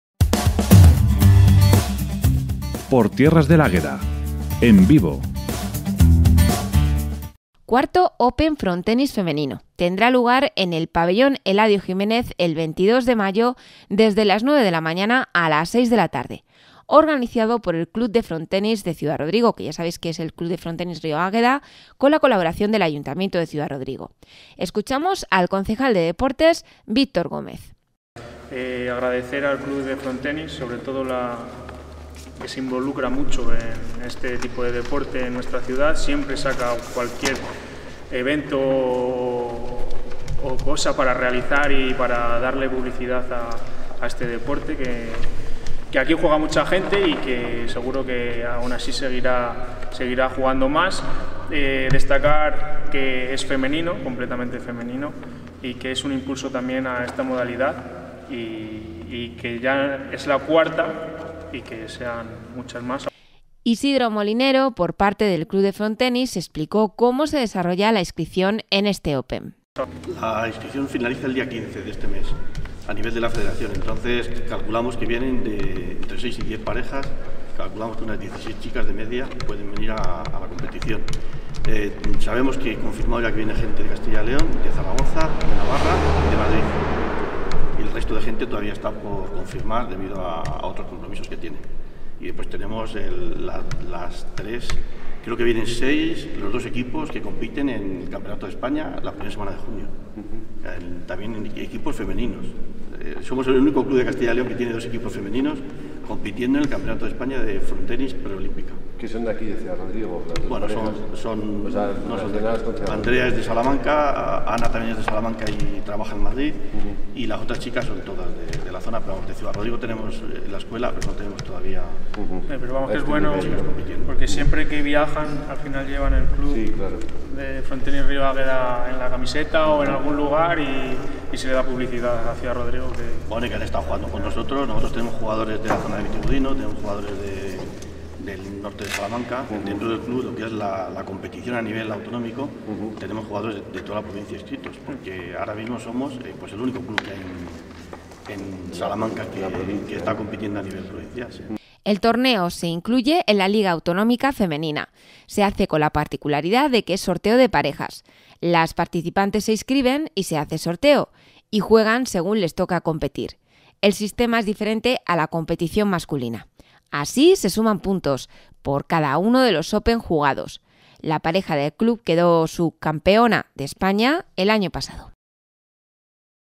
IV Open Frontenis Femenino
Pabellón Eladio Jimenez de 9 a 6 de la tarde organizado por el club de Frontenis con la colaboración del ayuntamiento DE Ciudad Rodrigo, escuchamos al concejal de deportes Victor Gómez